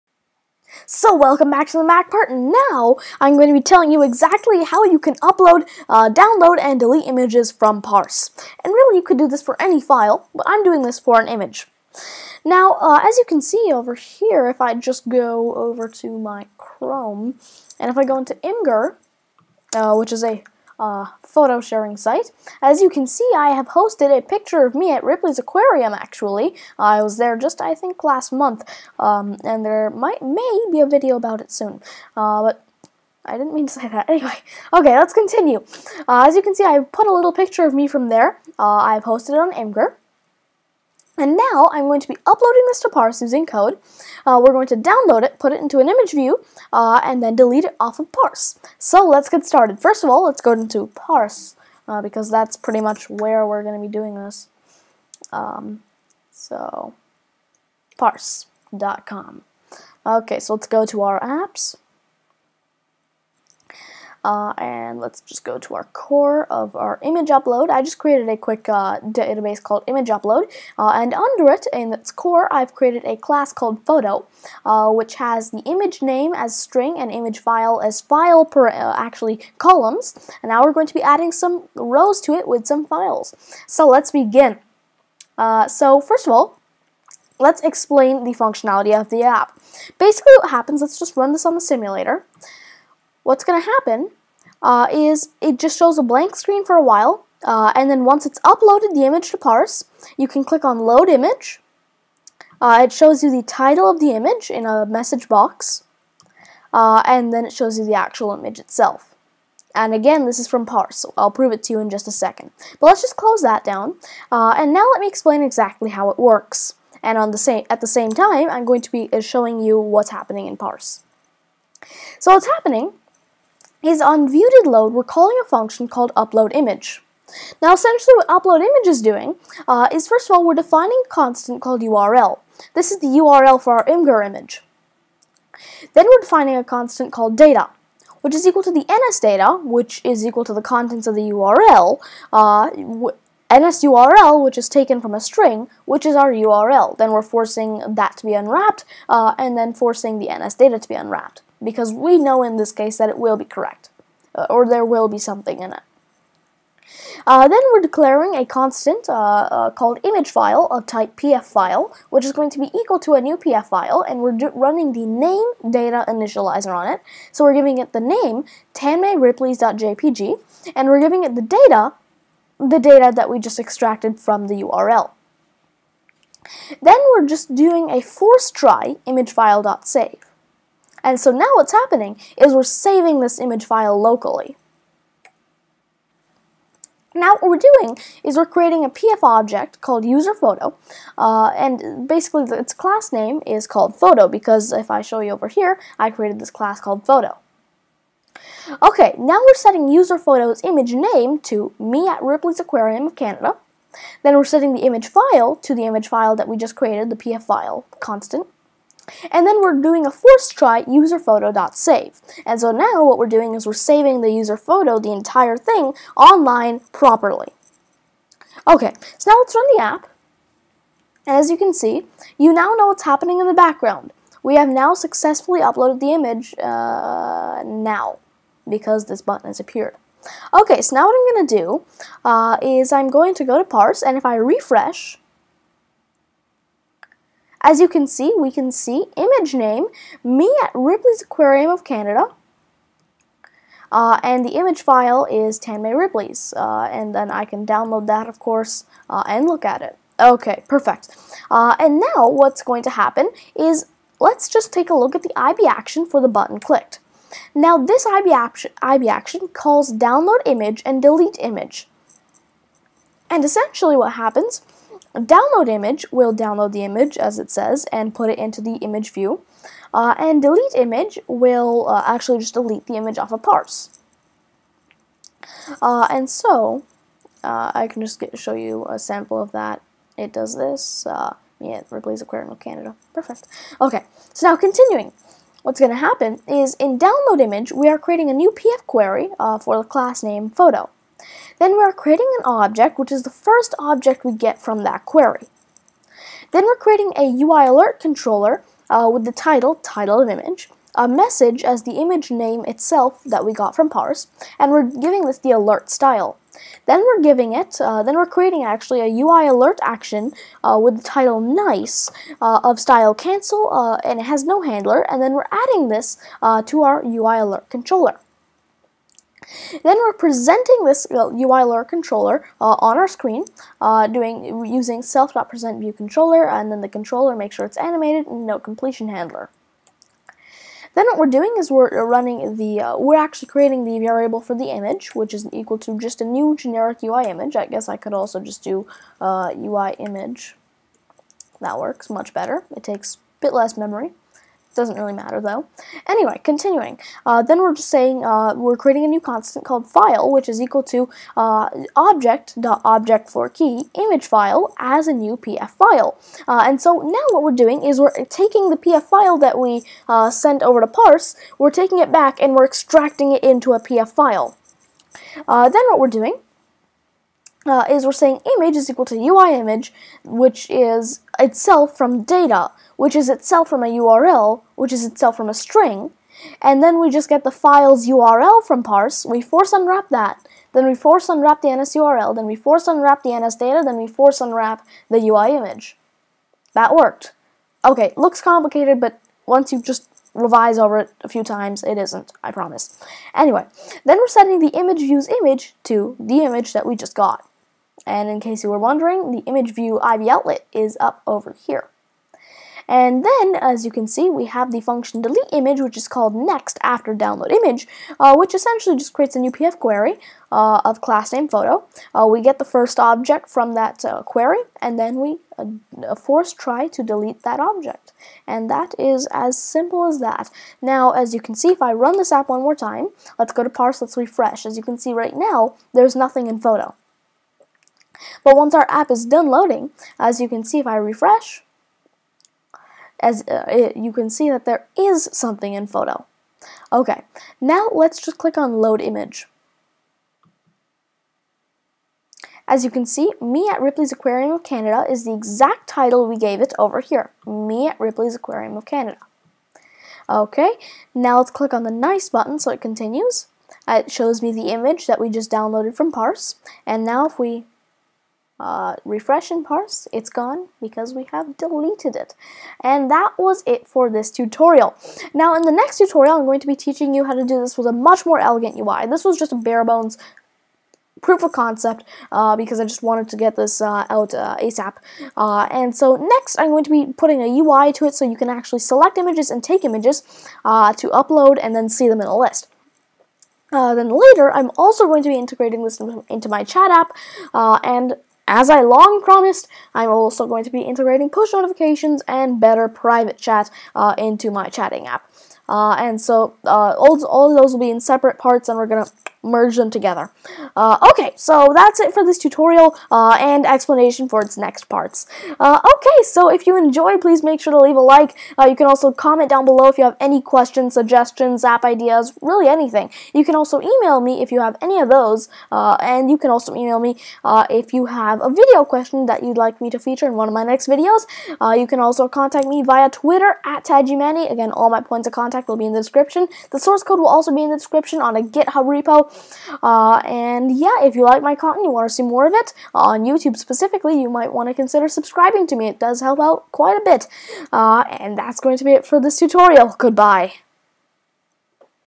Swift Databases: Uploading, Downloading & Deleting Photos on Parse in Swift 2! (The Implementation)
In this tutorial, I covered the bare-bones of how to upload photos to Parse in Swift 2!

Anyway, I hope you enjoyed this tutorial!

Links & PoC:
Code: Code link here **INSERT

Bitcoin Address for Tips: 1HFvjkL571LbctmYodBFkg1HRGGQrVDNC5